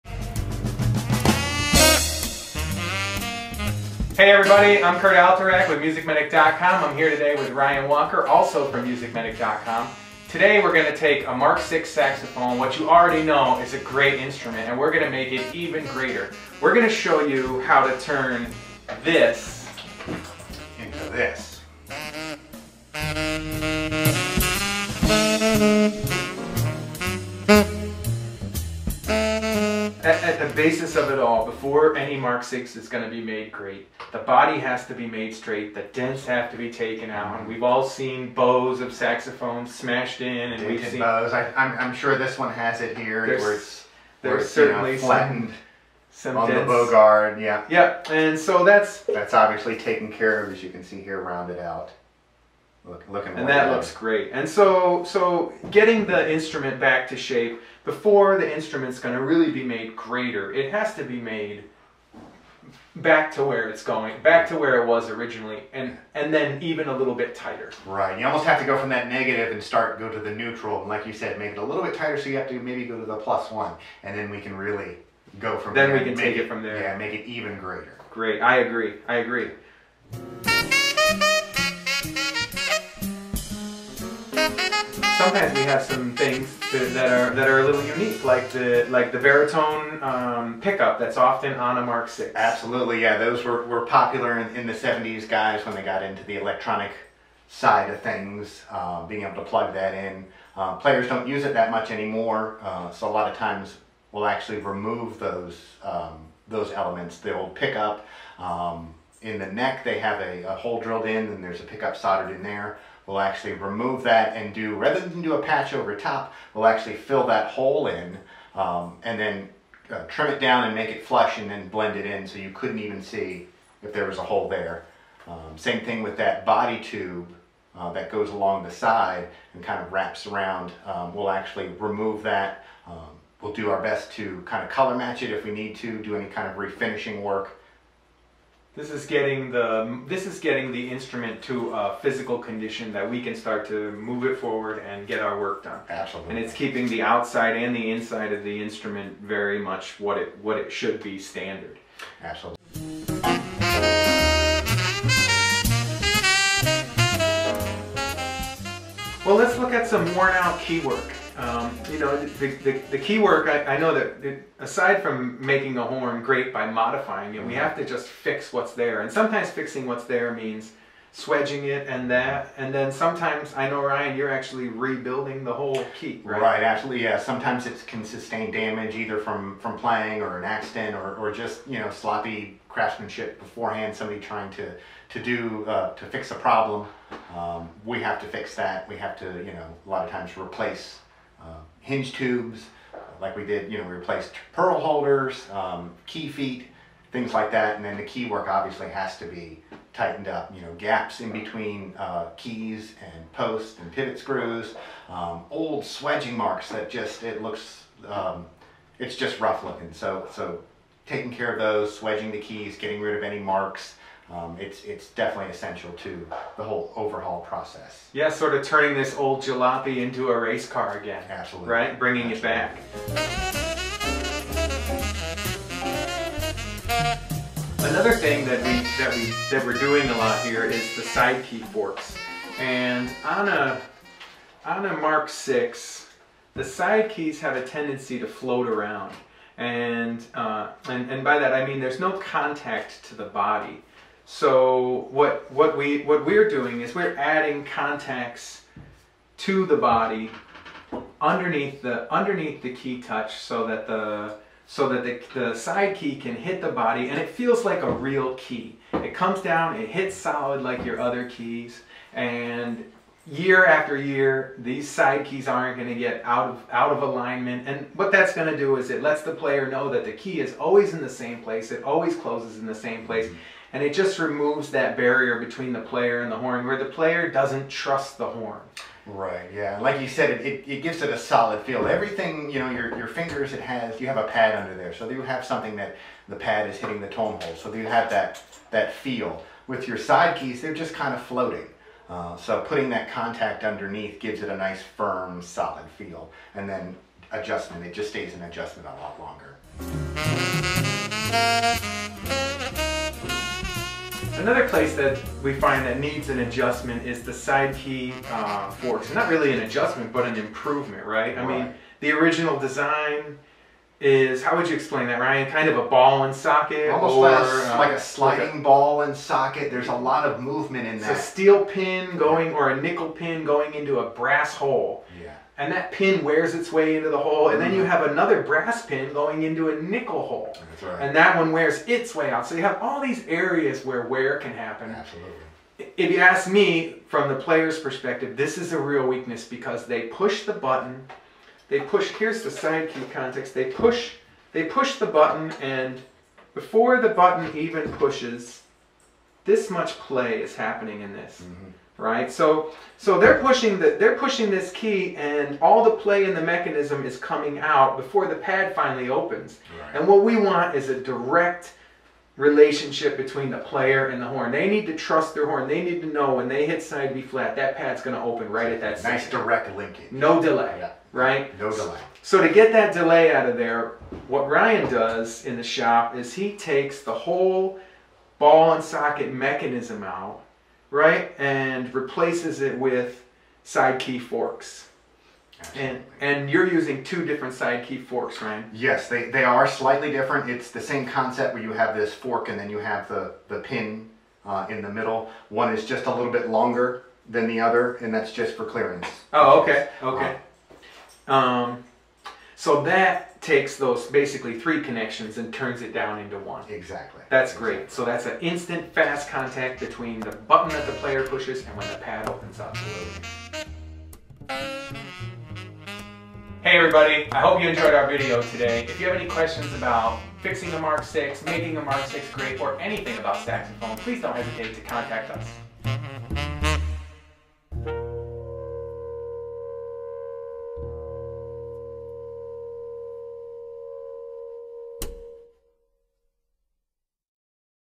The Complete Mark VI Überhaul | Part 2 of 4
In this 4-part series, we'll give an overview of a complete overhaul of the Selmer Mark VI saxophone. Join us as Curt and Ryan take you on a technician's journey into the world's most sought-after horn!

Topics covered in this part 2 of 4 include:
Body Dents
Veritone Pick-up
Warn Out Key Work
Side Key Fork
Side Key Ball Joint